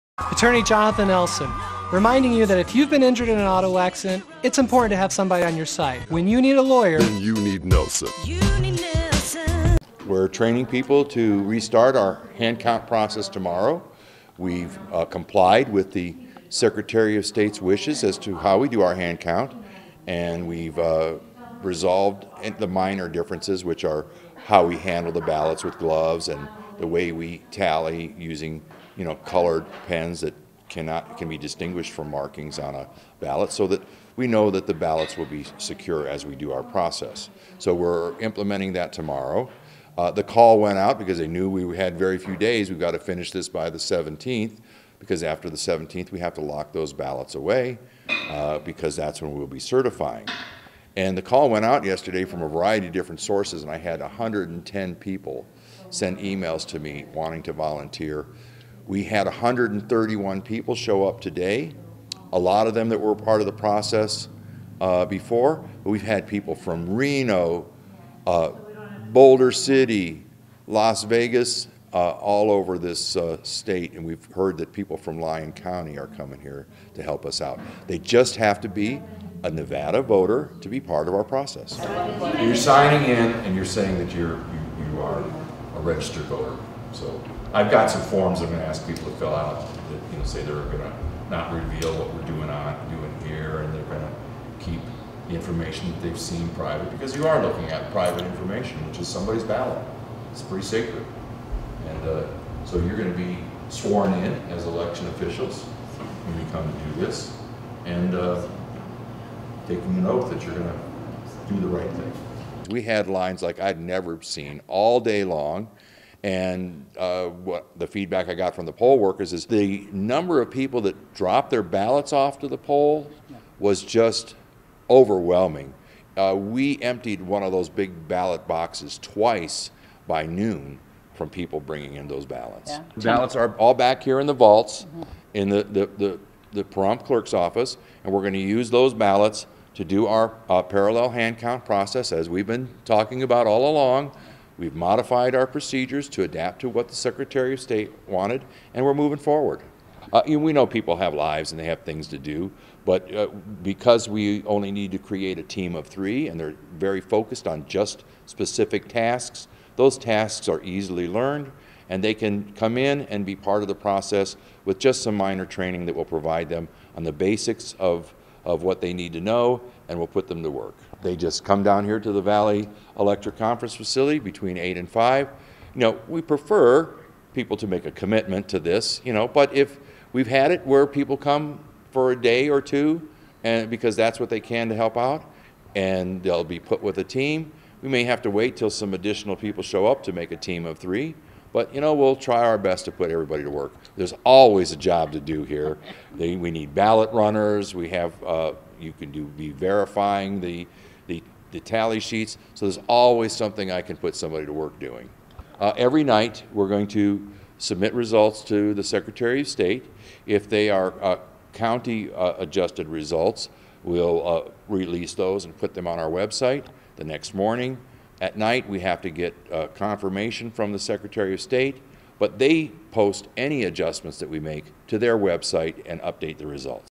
11/10/2022 Volunteers Begin Counting Paper Ballots for the General Election
Nye County Clerk Elect Mark Kampf has been busying training and now counting the paper ballots that were the main part of his campaign platform during his successful election. After running the paper ballots thru the machine now volunteers are hand counting them at the Valley Electric conference center to confirm the machine is correct. As in every election cycle mail in ballots are still coming in the mail. The vote cannot be certified until the 18th of November. The hand count must be completed by the 17th to send it up to the state.